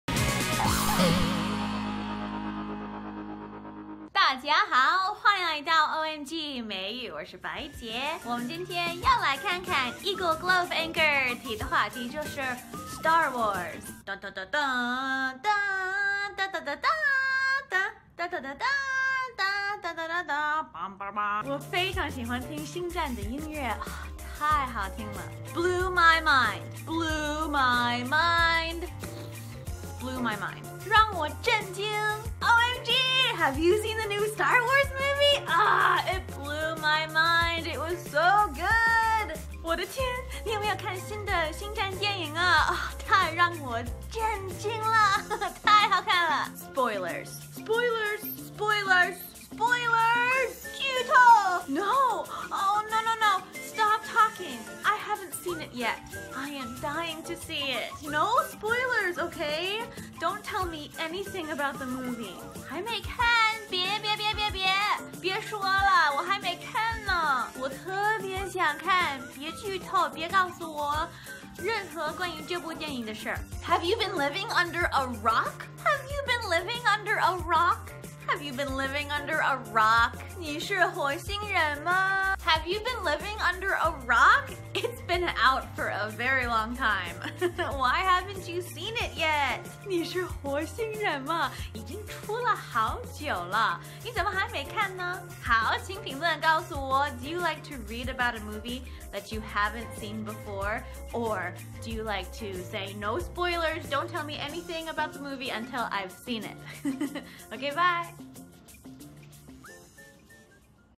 OMG!美语 Star War 2
今天我们来看看@EagleGlobeAnchor 提的话题：Star Wars!

blew my mind 让我震惊
OMG! Have you seen the new Star Wars movie!? It blew my mind it was so good!!!

我的天！你有没有看新的星战电影！？太让我震惊了！太好看了！！！
spoilers 剧透
No! Oh no no no! Stop talking. I haven't seen it yet. I am dying to see it. No spoilers.